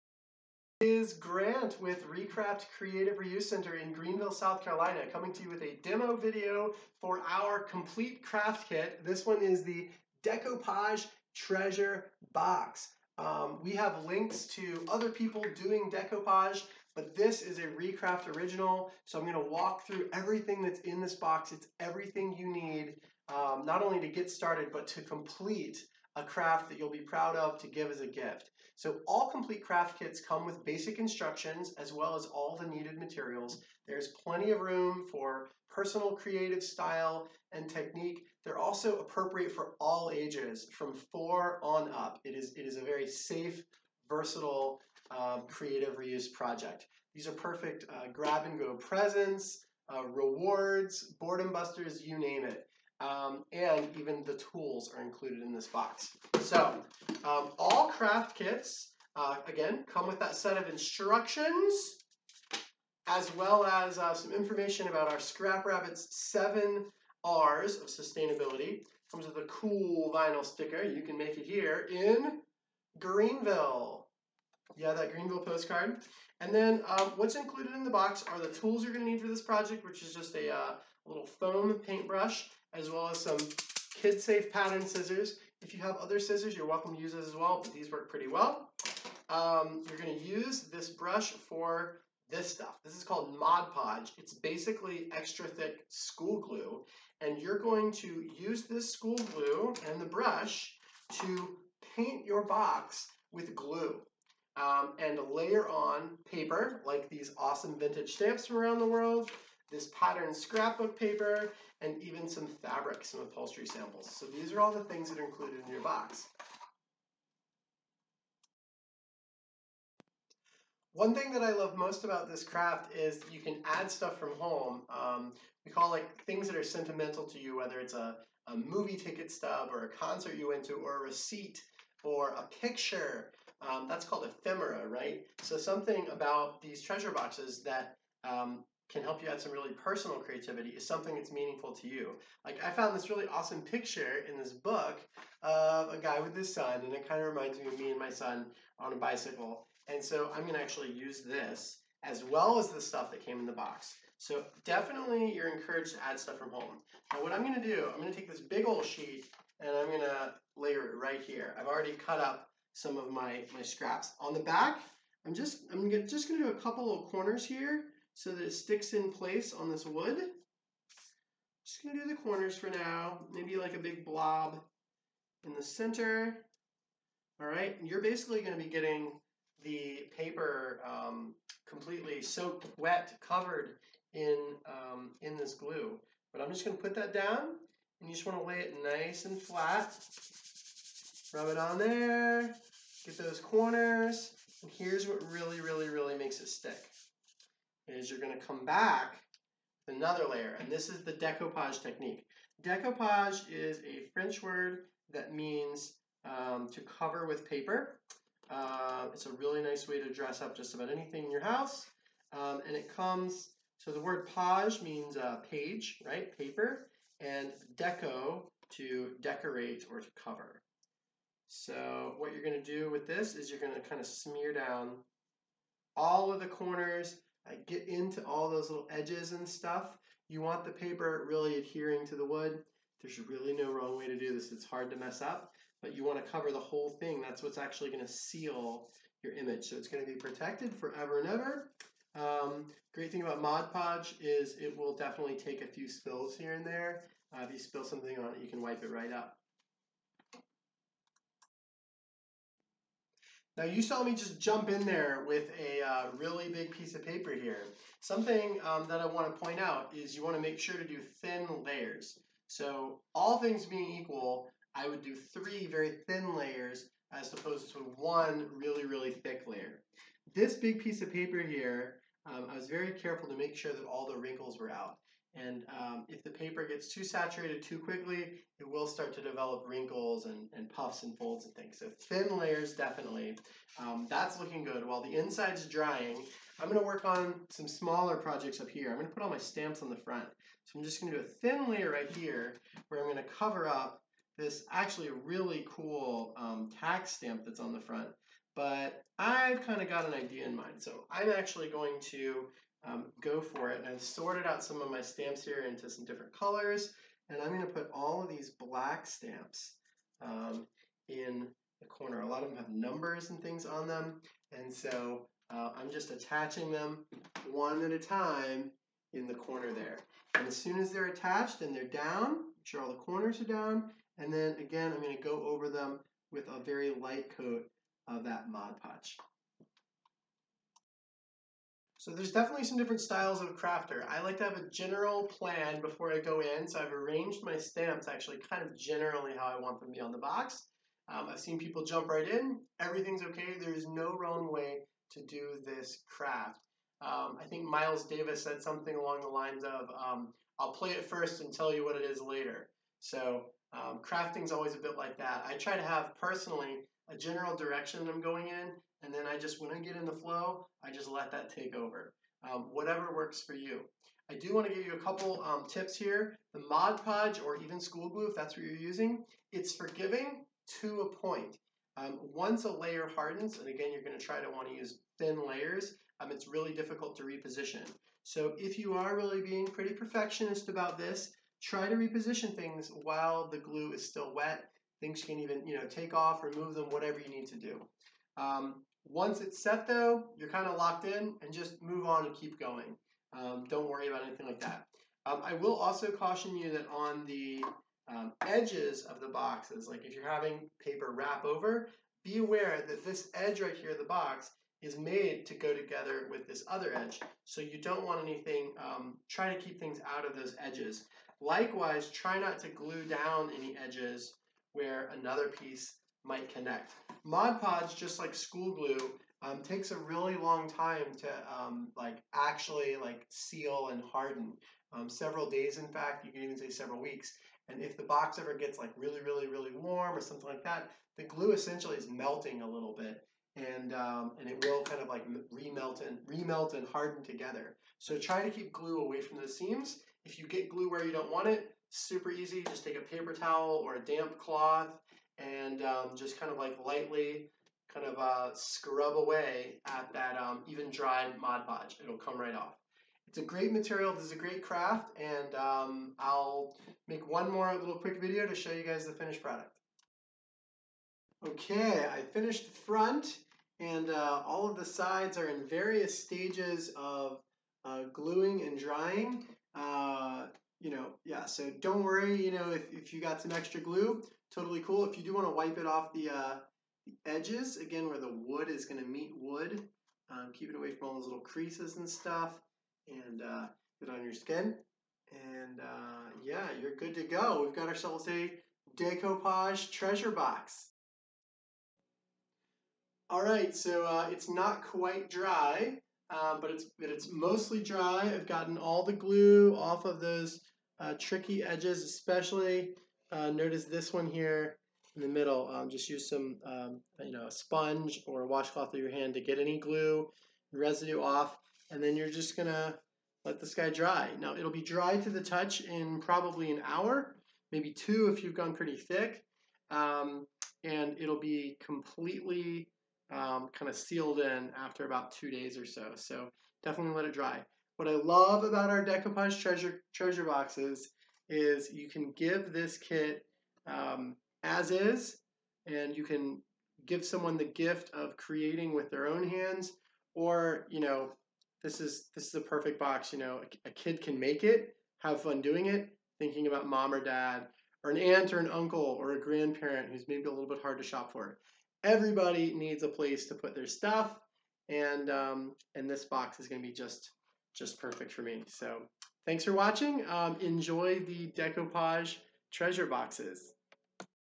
Creative Reuse Demo - Decoupage Treasure Box Complete Craft Kit
No sewing required! Make an eco-friendly bag you'll be proud to have on your arm. Each kit contains a brand-new, super-soft XS t-shirt from our friends at Swamp Rabbit Cafe -- and instructions for its transformation.